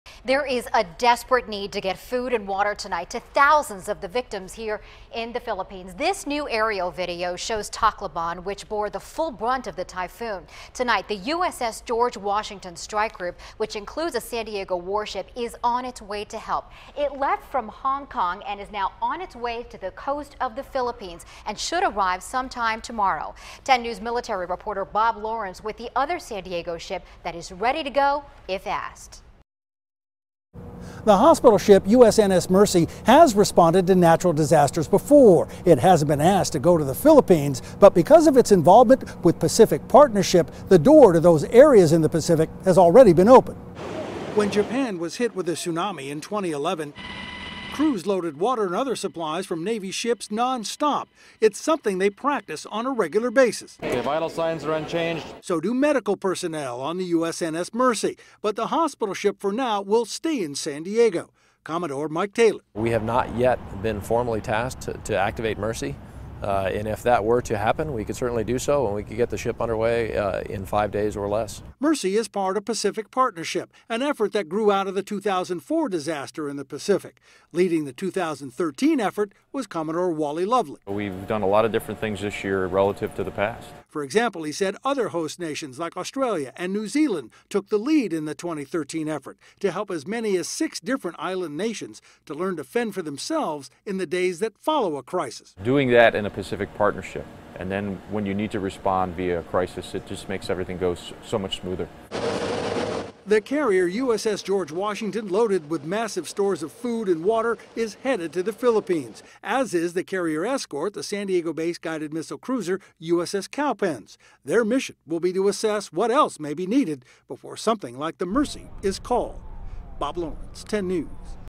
Hospital ship USNS Mercy on standby, other SD-based ship USS Cowpens headed to Philippines
A San Diego-based warship, the guided missile cruiser USS Cowpens, is headed to the Philippines with an aircraft carrier strike group to help in the wake of a devastating typhoon.